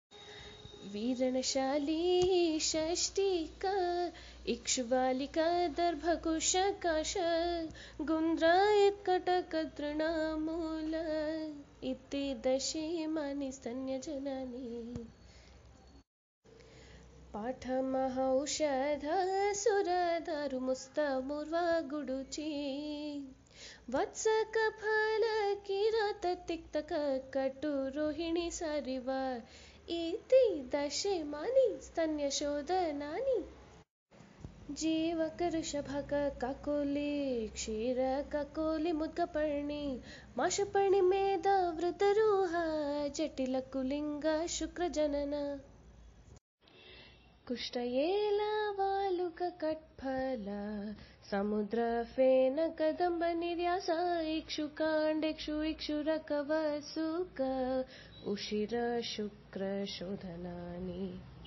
Charaka ganas 17 to 20 - tunes to shlokas - easy to memorise
Charaka ganas by SUVACHA BATCH, GAMC BENGALURU - Sthanya janana, Sthanya shodhana, Shukra janana and Shukra shodhana